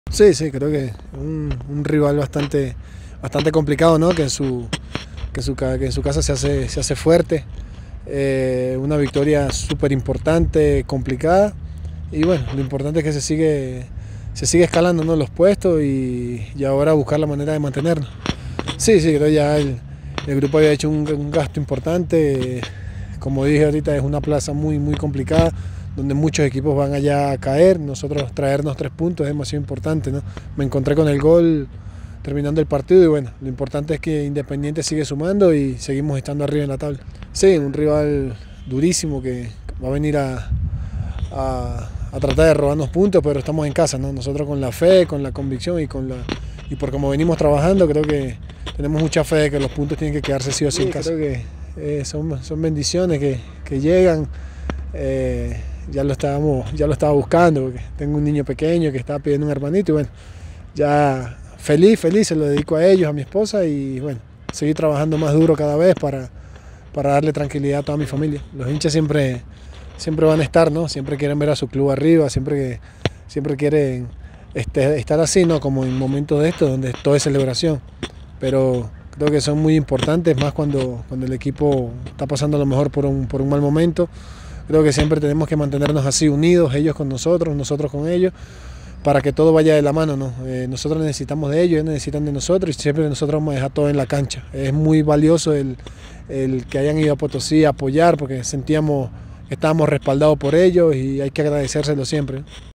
MIJAIL AVILÉS INDEPENDIENTE PETROLERO QUE AHORA ES SEGUNDO EN LAS POSICIONES DEL FÚTBOL BOLIVIANO
Video gentileza Prensa Independiente Petrolero